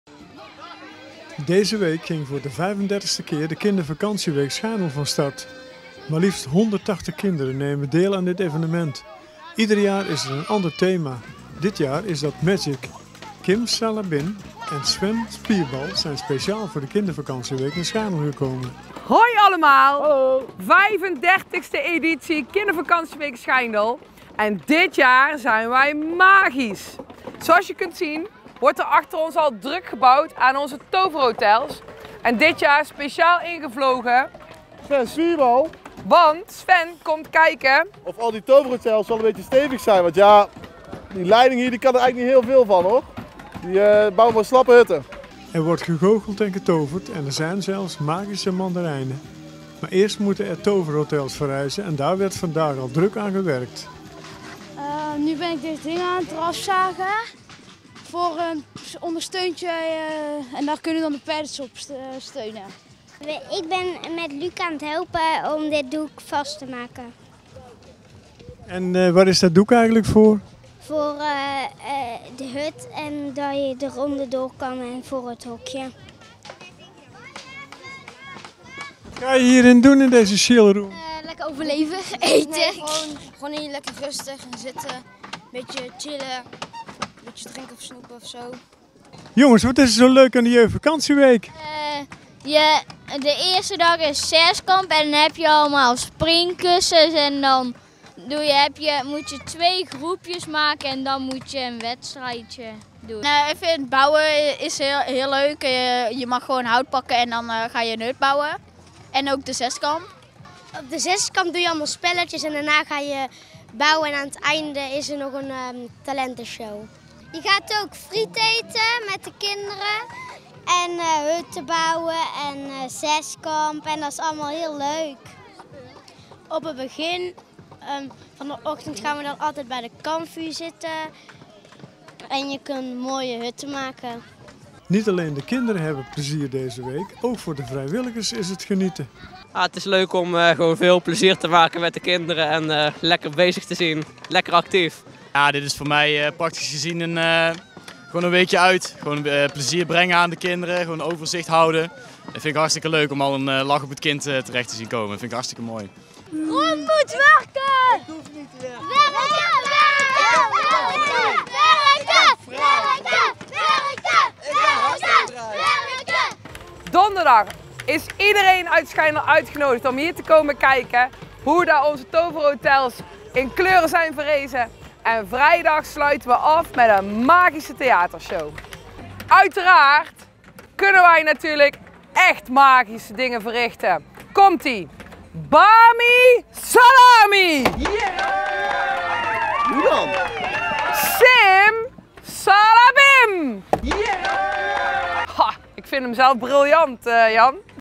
20190716 Kindervakantieweek Schijndel
Schijndel - Deze week ging voor de 35ste keer de kindervakantieweek Schijndel van start. Maar liefst 180 kinderen nemen deel aan dit evenement. Ieder jaar is er een ander thema. Dit jaar is dat magic. Er wordt gegoogeld en getoverd en er zijn zelfs magische mandarijnen.  Maar eerst moeten er toverhotels verrijzen en daar werd vandaag al druk aan gewerkt. Kim Salabim en Sven Spierbal zijn special voor de kindervakantieweek naar Schijndel gekomen. Sven (Joppe van den Broek) is gekomen om de stevigheid van de bouwsels in de gaten te houden. Kim (Rian van der Schoot) is goed in googelen.  Speciaal voor Omroep Meierij laat zij met de bijzondere toverspreuken “Bami Salami” en “Sim Salabim” een heel servies naar beneden vallen.
Jidde, Julius, Hidde, Sacha en Douwe genieten vooral van het hutten bouwen tijdens deze kindervakantieweek. Sacha vind het frietjes eten met de anderen ook heel leuk terwijl Douwe graag ‘s morgens rond het kampvuur zit. Donderdag mogen opa’s, oma’s, pappa’s en mamma’s, broertjes en zusjes komen kijken naar wat er zoal gebouwd is en natuurlijk naar de kleurige toverhotels. Vrijdag wordt de week afgesloten met een magische theatershow, aldus Kim.